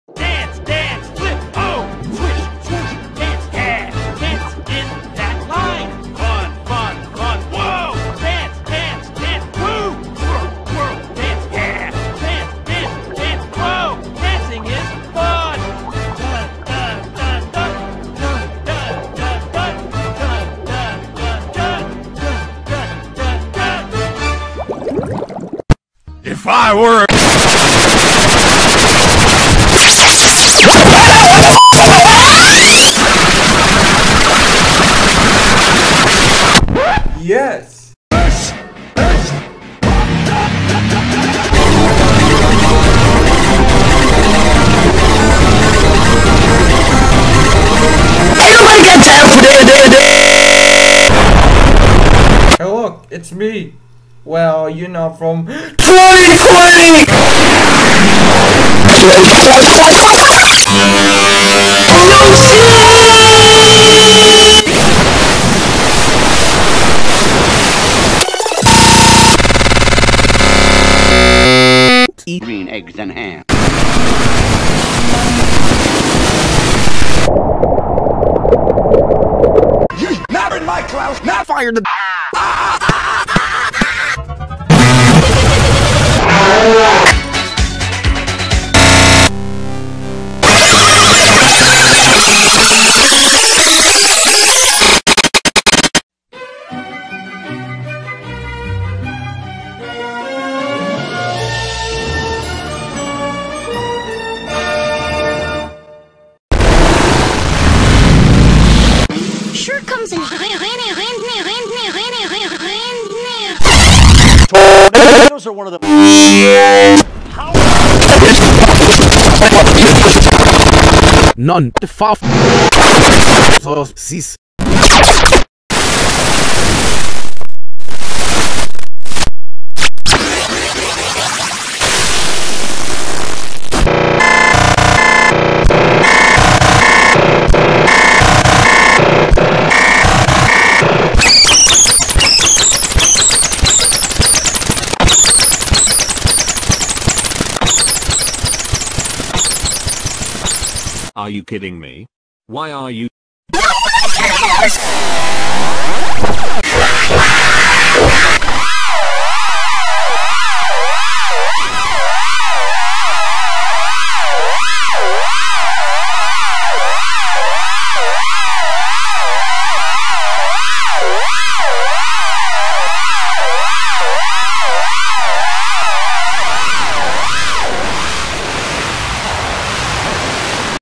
[YTP TENNIS] The long waited Round 37 is here
long waited Round 37 vs EmilianoYTP, CubeyCarter and Everyone on The Kukori YTP Tennis Match